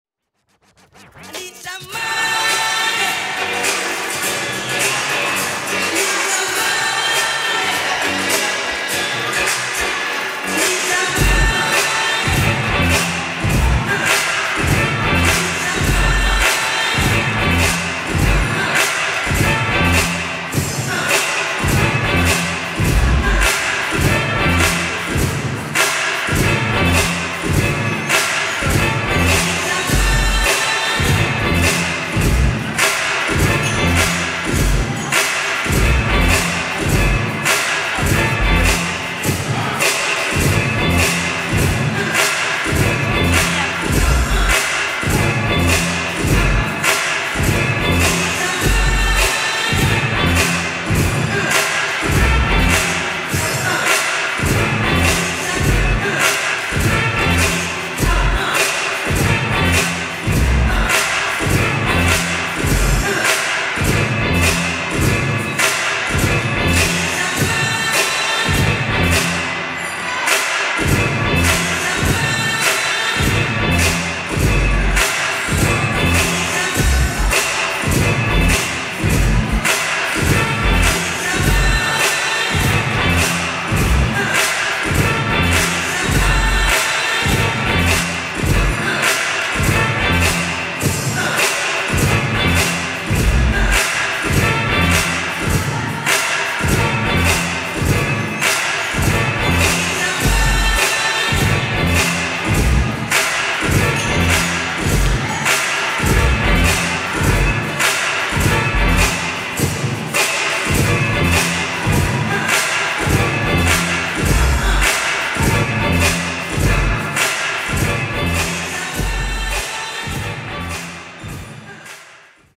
🏆 WinterCup2023 🏆🔥Чемпионат по современным танцам 🔥
🔥Чемпионат по современным танцам 🔥

🗓️Даты проведения: 25-26 февраля
📍Место проведения: г. Молодечно, ул. Великий Гостинец, 102, «Спортивно- развлекательный центр «Олимпик-2011»

Танцевальные направления:
⭐️Хип-хоп